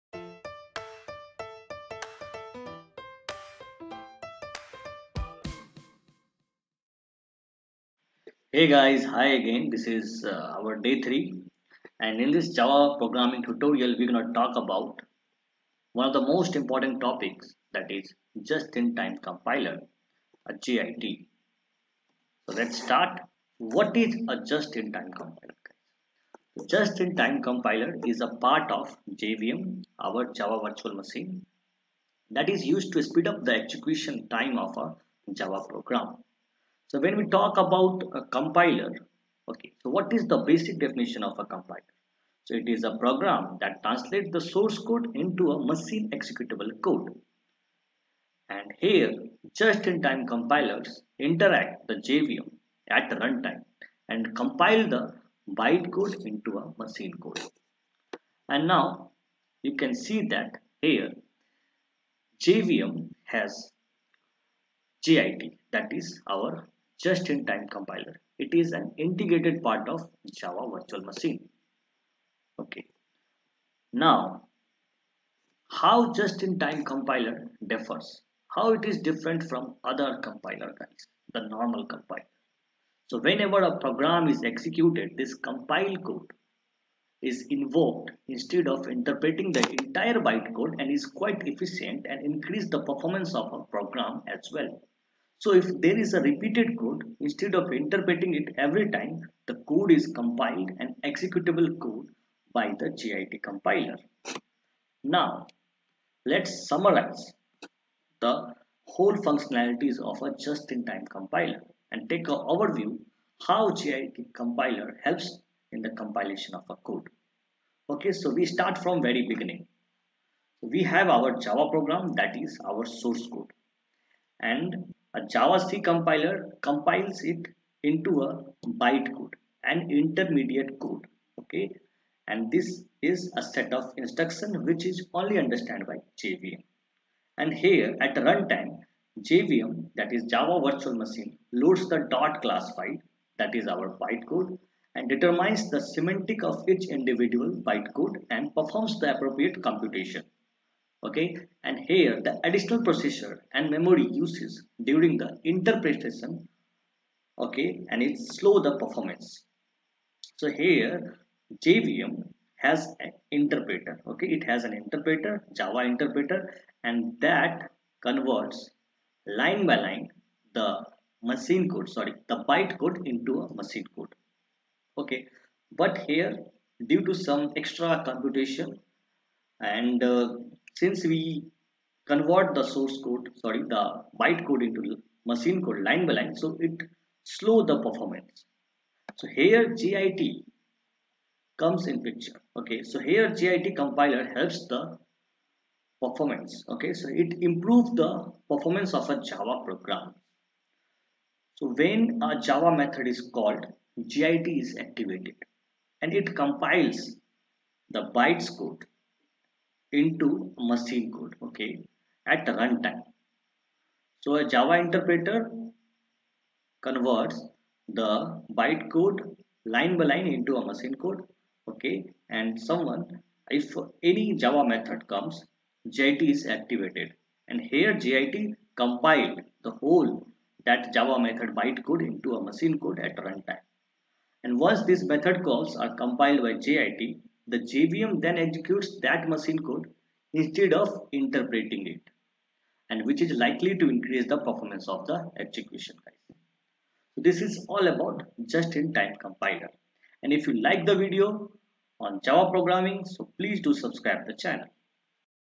Java Tutorial For Beginners | Just in time (JIT) Compiler | Cyber Ocean Academy!
🔥 This Cyber Ocean Academy!  video on "Java Programming tutorial for beginners

Lecture - 03

what is JIT?
Just in time compilelr (JIT)  of JVM that is used to speed up the execution time of a program.
Basic definition of compiler is “a program that translates the source code to a machine executable code”.
 JIT compilers interact the JVM runtime and compiled bytecode into machine code.